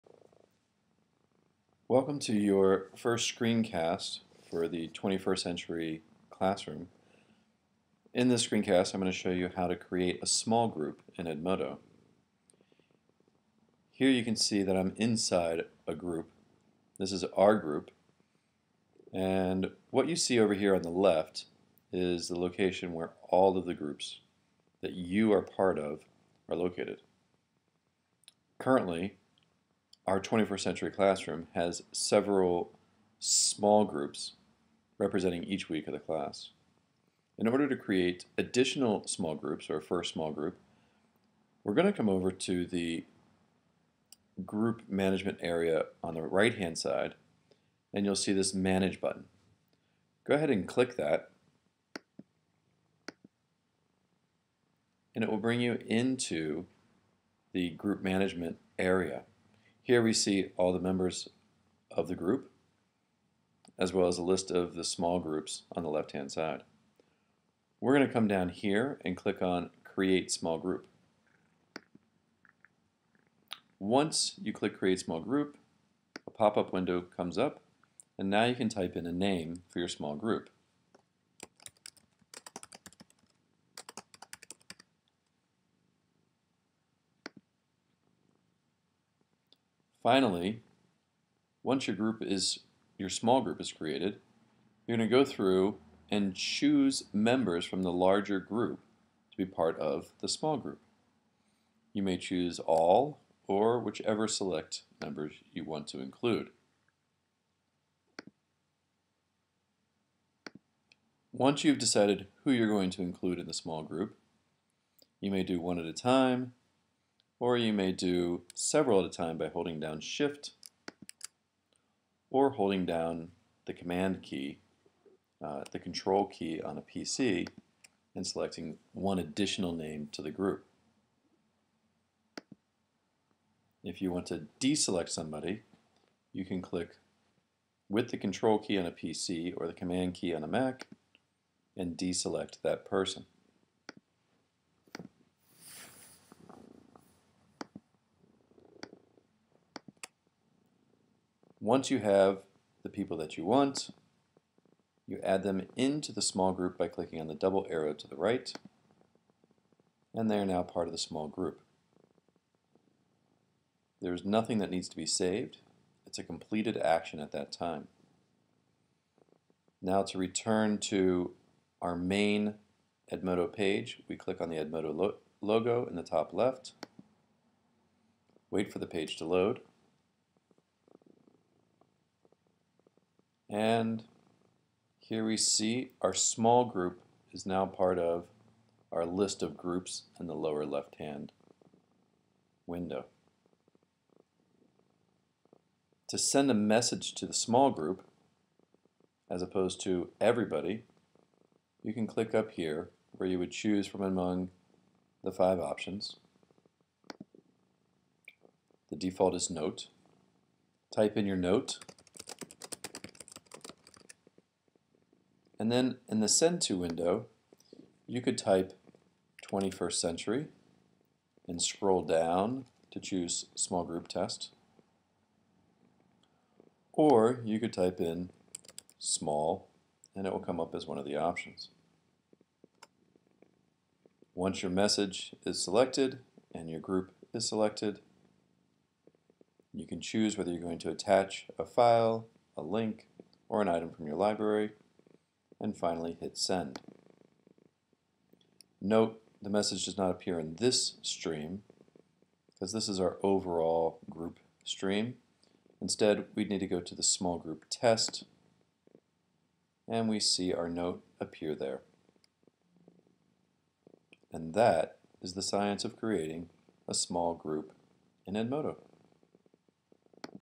Edmodo Small Group Setup
Chad demonstrates how to create a small group within a group using Edmodo.com